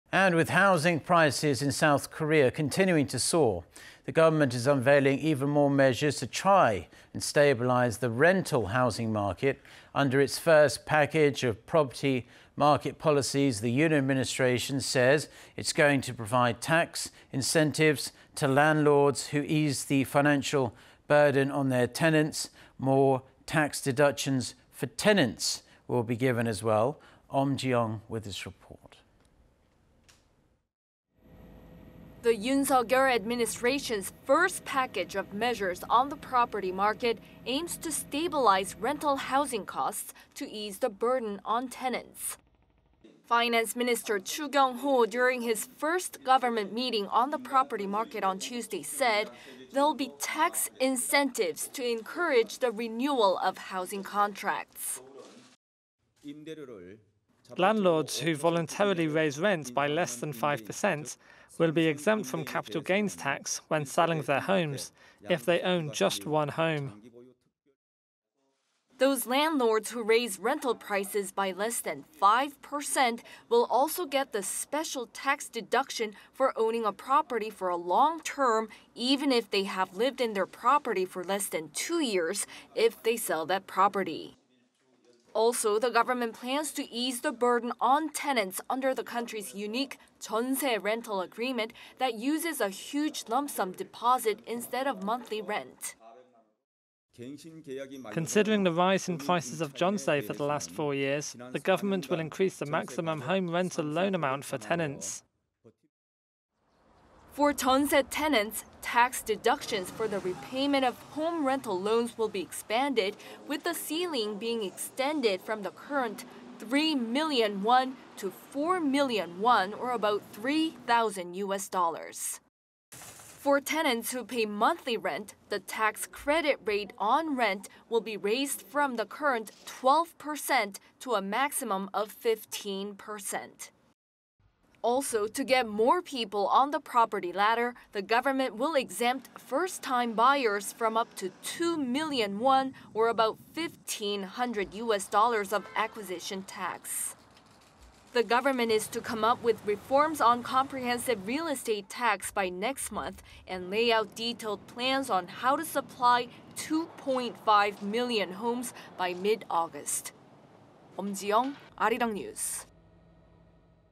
S. Korea to stabilize rental housing market through tax cuts, other measures
추경호 "5% 이내 인상 '상생임대인' 양도세 비과세, 거주조건 면제...생애 첫 주택 취득세 200만 원 면제"

With housing prices in South Korea continuing to soar, the government is unveiling even more measures to try and stabilize the rental housing market.
Under its first package of property market policies, the Yoon administration says it will provide tax incentives to landlords who ease the financial burden on their tenants.
More tax deductions for tenants will be given as well.
Eum Ji-young has more.

The Yoon Suk-yeol administration's first package of measures on the property market aims to stabilize rental housing costs to ease the burden on tenants.
Finance Minister Choo Kyung-ho, during his first government meeting on the property market on Tuesday, said there will be tax incentives to encourage the renewal of housing contracts.

"Landlords who voluntarily raise rent by less than 5 percent will be exempt from capital gains tax when selling their homes if they own just one home."

Those landlords who raise rental prices by less than 5 percent, will also get the special tax deduction for owning a property for a long-term even if they have lived in their property for less than two years if they sell that property.
Also, the government plans to ease the burden on tenants under the country's unique 'jeonse' rental agreement that uses a huge lump-sum deposit instead of monthly rent.

"Considering the rise in prices of 'jeonse' for the last four years, the government will increase the maximum home rental loan amount for tenants."

For 'jeonse' tenants, tax deductions for the repayment of home rental loans will be expanded,... with the ceiling being extended from the current three million won to four million won or about 3-thousand U.S. dollars.
For tenants who pay monthly rent, the tax credit rate on rent will be raised from the current 12 percent to a maximum of 15 percent.
Also, to get more people on the property ladder, the government will exempt first time buyers from up to 2 million won, or about 15-hundred U.S. dollars of acquisition tax.
The government is to come up with reforms on comprehensive real estate tax by next month and lay out detailed plans on how to supply 2-point-5 million homes by mid August.
Eum Ji-young, Arirang News.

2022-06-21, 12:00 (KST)